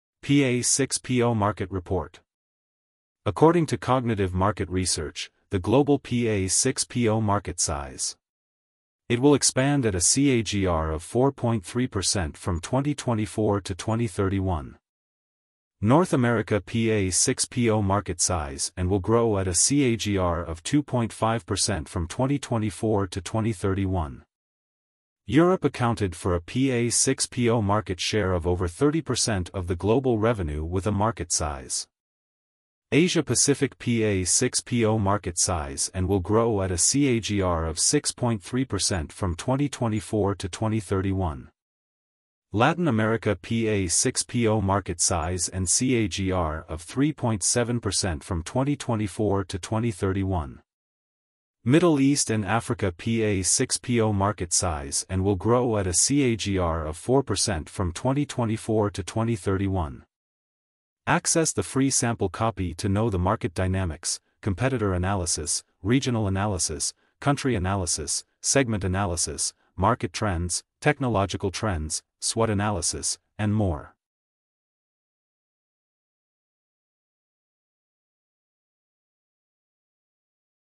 PA6PO Market Report 2024 | Forecast, Market Size, Growth, Trends
Report Insights: According to Cognitive Market Research, the global PA6PO market size will be USD XX million in 2024. It will expand at a compound annual growth rate (CAGR) of 4.3% from 2024 to 2031.

North America held the major market share for more than 40% of the global revenue with a market size of USD XX million in 2024 and will grow at a compound annual growth rate (CAGR) of 2.5% from 2024 to 2031.
Europe accounted for a market share of over 30% of the global revenue with a market size of USD XX million.
Asia Pacific held a market share of around 23% of the global revenue with a market size of USD XX million in 2024 and will grow at a compound annual growth rate (CAGR) of 6.3% from 2024 to 2031.
Latin America had a market share of more than 5% of the global revenue with a market size of USD XX million in 2024 and will grow at a compound annual growth rate (CAGR) of 3.7% from 2024 to 2031.
Middle East and Africa had a market share of around 2% of the global revenue and was estimated at a market size of USD XX million in 2024 and will grow at a compound annual growth rate (CAGR) of 4.0% from 2024 to 2031.